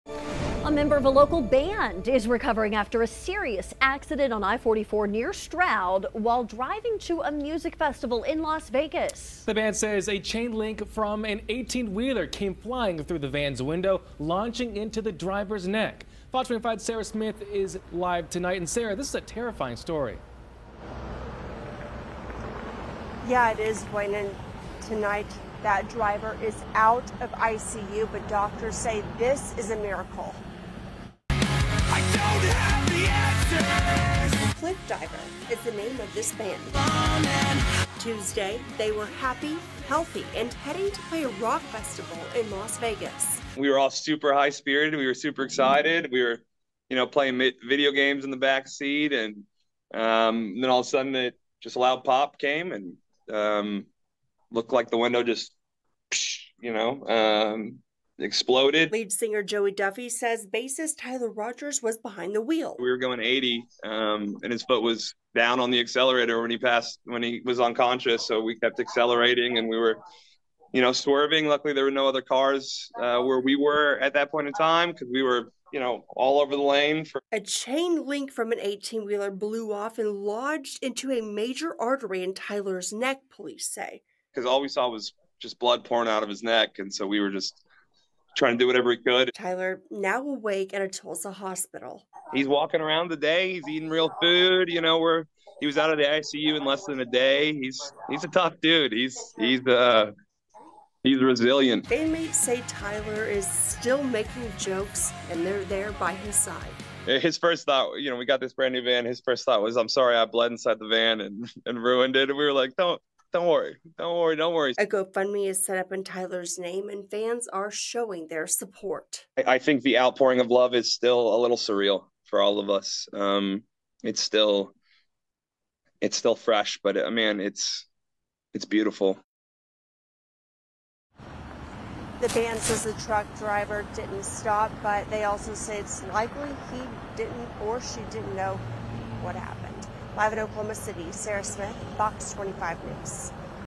CLIFFDIVER band member recovering after chain link lodges into neck while driving to music festival
A MEMBER OF A LOCAL BAND IS RECOVERING -- AFTER A SERIOUS ACCIDENT ON I-44 NEAR STROUD. WHILE DRIVING TO A MUSIC FESTIVAL IN LAS VEGAS.
THE BAND SAYS A CHAIN LINK FROM AN 18 WHEELER CAME FLYING THROUGH THE VAN'S WINDOW -- LAUNCHING INTO THE DRIVER'S NECK.
TONIGHT...THE DRIVER IS OUT OF ICU..DOCTORS SAYING ITS A MIRACLE HE IS ALIVE.

nats of video
CLIFFDIVER IS THE NAME OF THIS BAND...
nats of video

TUESDAY...THEY WERE HAPPY... HEALTHY...AND HEADING TO PLAY A ROCK FESTIVAL IN VEGAS.

02:39 we were all super excited. In high spirits. We were playing video games in the back seat and all of a sudden this loud pop came and the window blew out and exploded. 02:55

LEAD SINGER JOEY DUFFY SAYS BASSIST TYLER ROGERS WAS BEHIND THE WHEEL.

3:41 we were going 80 and his foot was down on the accelerator when he was unconscious. We kept accelerating and there were no other cars where we were because we were all over the lanes. 3:52
JOEY DUFFY
CLIFFDIVER SINGER

A CHAIN LINK OFF AN 18 WHEELER BLEW OFF AND LODGED INTO A MAJOR ARTERY IN TYLERS NECK, POLICE SAY.
(picture of the chain)

5:12 all we saw was blood pouring out of his neck and we were just trying to do whatever we could. 5:16
JOEY DUFFY
CLIFFDIVER SINGER

TYLER - NOW AWAKE AT A TULSA HOSPITAL.

6:52 he is walking around today he is eating real food. He is out of the icu. He is a tough dude. He is resilient. 7:00
JOEY DUFFY
CLIFFDIVER SINGER

BANDMATES SAY TYLER IS STILL MAKING JOKES.
THEY'RE THERE BY HIS SIDE.

10:34 his first words when he woke up was I am sorry I bled inside the van and we were like do not worry! 10:41
JOEY DUFFY
CLIFFDIVER SINGER

A GO FUND ME IS SET UP IN TYLERS NAME AND FANS ARE SHOWING SUPPORT.

9:06 I think the outpouring of love is surreal. It is fresh but beautiful. 9:16JOEY DUFFY
CLIFFDIVER SINGER

THE BAND SAYS THE TRUCK DRIVER DIDN'T STOP. BUT ITS LIKELY THEY DIDN'T KNOW THIS HAPPENED.